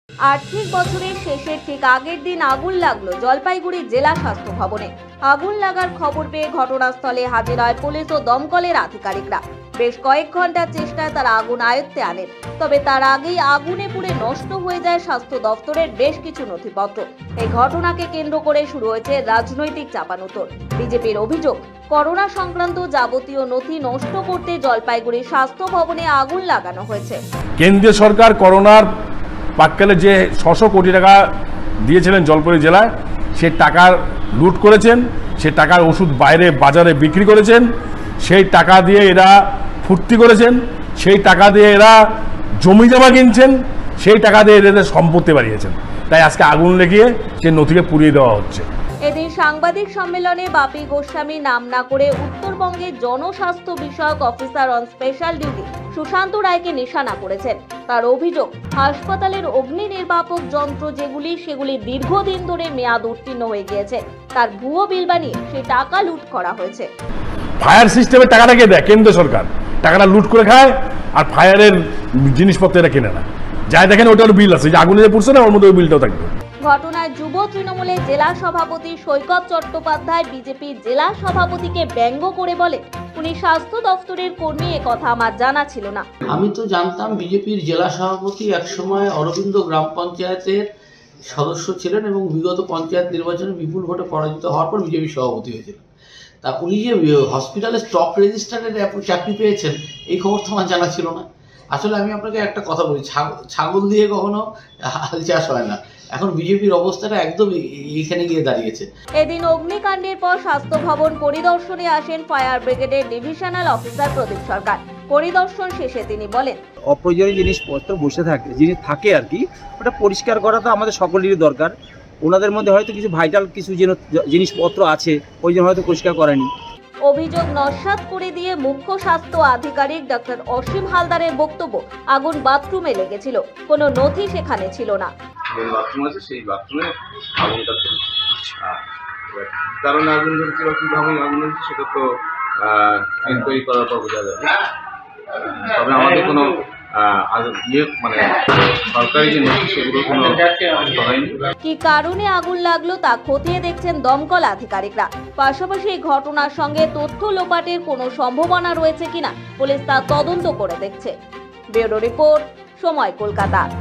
জলপাইগুড়ি জেলা স্বাস্থ্যভবনে আগুনপ্রমান লোপাটের অভিযোগ নিয়ে চাপানউতোর
জলপাইগুড়ি জেলা স্বাস্থ্যভবনে আগুন
প্রমান লোপাটের অভিযোগ নিয়ে চাপানউতোর